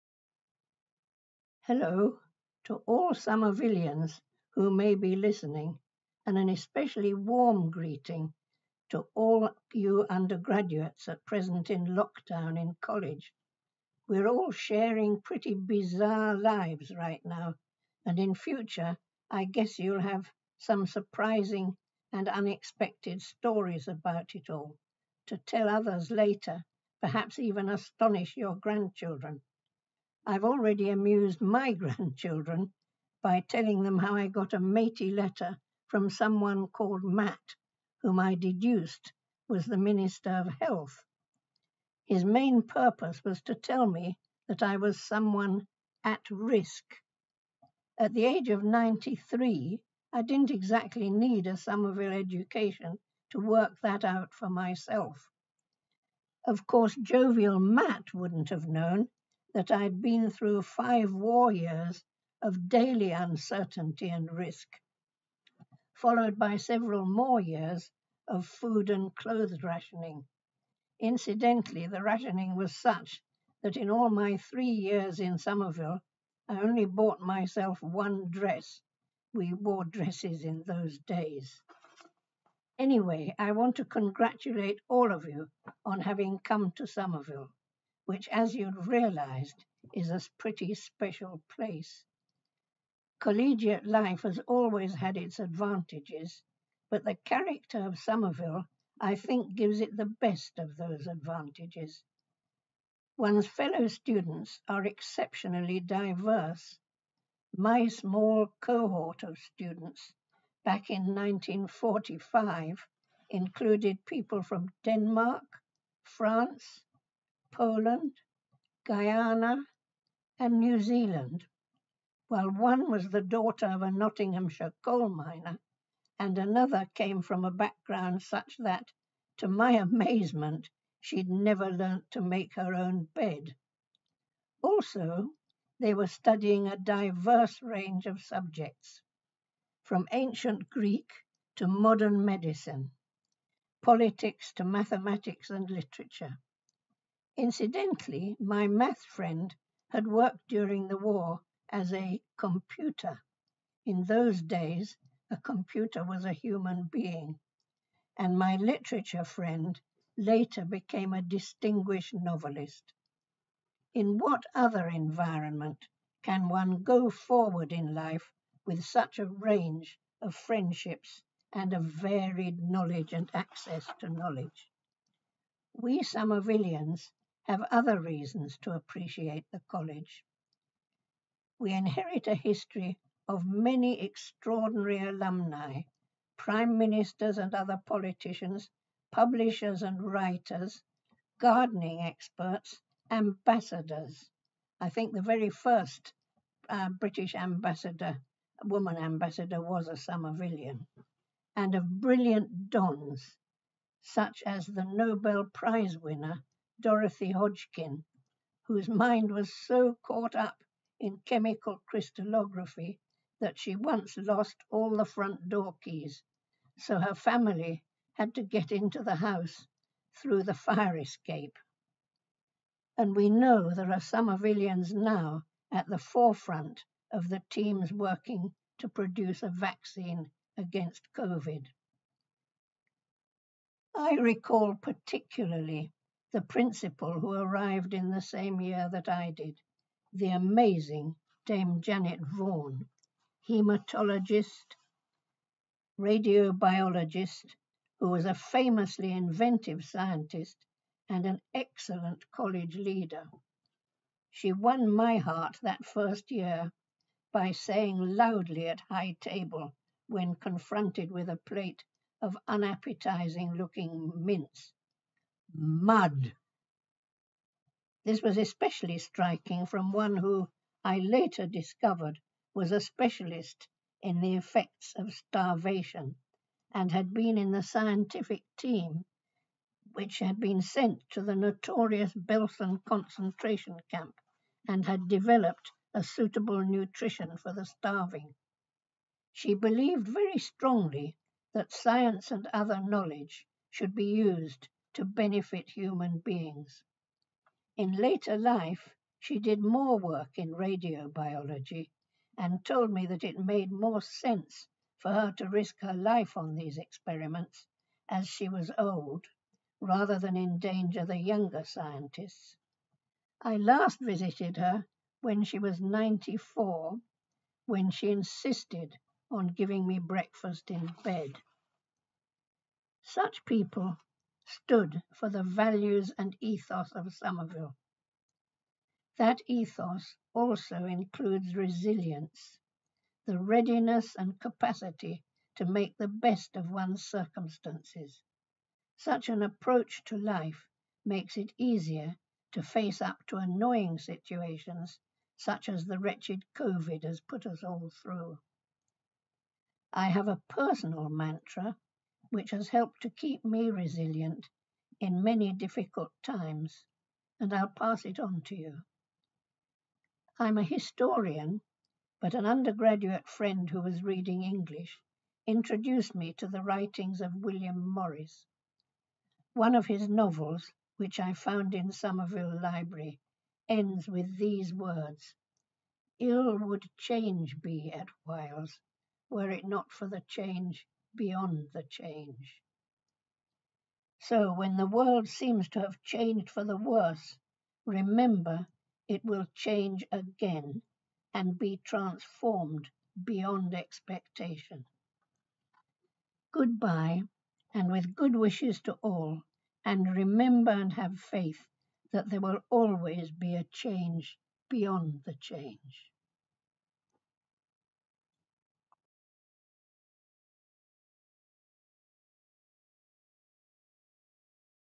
The Upside: Lalage Bown
93 year-old Lalage Bown received a 'matey' letter from a man called 'Matt' back in March, telling her that she was at risk due to Covid-19, something that - as she puts it, "you don't need a Somerville education to deduce" at her age.

Something else the Health Minister didn't realise is that, having lived through 5 years of WW2 and the years of rationing afterwards, Lalage knows how to navigate tough times.

Listen as Lalage shares her hard-earned wisdom on getting through and how change is always just around the corner - but, as she reminds our students, not to wish the days past but to keep treasuring each friendship.

Lalage studied History at Somerville in 1945. She then embarked on a pioneering career developing adult education and literacy programs, particularly in Africa, where she also helped to Africanise curricula (including publishing one of the first surveys of African non-fiction, 'Two Centuries of African English') and organise the first academic conference on African culture to be held on African soil. She was recognised for her work by an honorary doctorate by the Open University, an OBE, and Fellowship of the Royal Society of Edinburgh, among many honours.